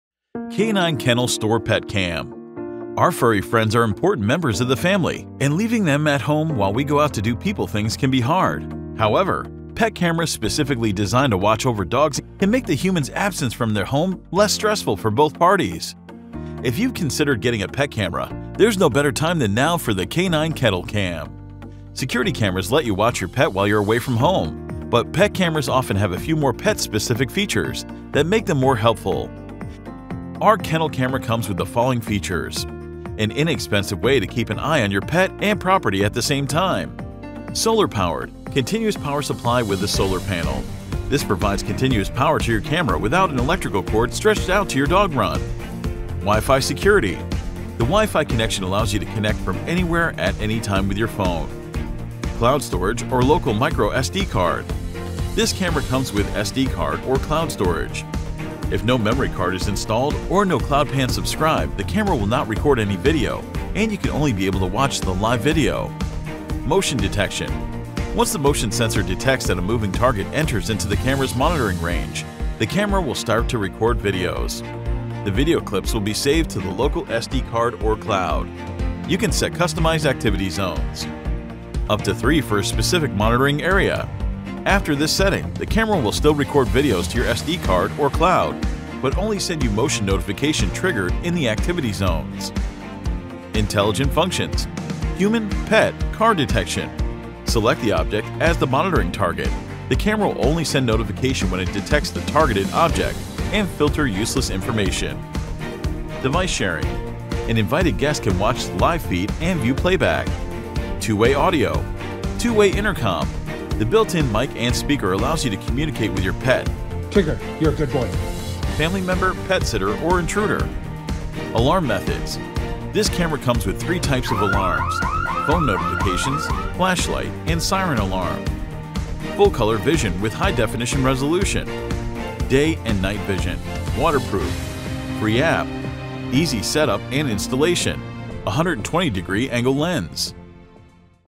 K9 Kennel Store Pet Cam
Our furry friends are important members of the family, and leaving them at home while we go out to do people things can be hard. However, pet cameras—specifically designed to keep watch over dogs and cats—can make the human's absence from the home less stressful for both parties. If you've considered getting a pet camera, there's no better time than now then the K9 Kennel Cam. Security cameras let you watch your pet while you're away from home, but pet cameras often have a few more pet-specific features that can make them more helpful. K9 Kennel Cam can help address common issues like separation anxiety and boredom, which can lead to destroyed furniture, other kinds of property damage, or undue stress.
Our Kennel Camera comes with the following features:
• An inexpensive way to keep an eye on your pet and property at the same time.
• Solar Powered: Continuous power supply with a solar panel. This provides continuous power to your camera without an electrical cord stretched out to your dog run.
• Wi-Fi security: The Wi-Fi connection allows you to connect from anywhere at any time with your phone.
• Cloud Storage or Local micro-SD card: This camera comes with SD card or cloud storage.
• Motion detection: Once the motion sensor detects that a moving target enters into the camera’s monitoring range the camera will start to record videos.  The video clips will be saved to the local SD card or cloud.
• Area Monitoring: You can set customized activity zones
•  up to 3 for a specific monitoring area. After this setting, the camera will still record videos to your SD card or cloud, but only send you motion notification triggered in the activity zones.
• Intelligent Functions: Human – Pet – car detection. Select the object as the monitoring target the camera will only send notification when it detects the targeted object and filter useless information.
• Device sharing: An invited guest can watch live feed and view playback
• Two Way Audio. Two way intercom, The built in MIC and Speaker allows you to communicate with your pet, family member, pet sitter or intruder.
• Alarm Methods: This camera comes with 3 types of alarms, Phone Notifications, Flashlight & Siren Alarm.
• Full Color Vision with High definition resolution
• Day and Night Vision
• Waterproof
• Free APP
• Motion sensor or motion triggered Recording & Manual recording
• Easy Setup and Installation
• 120-degree angle lens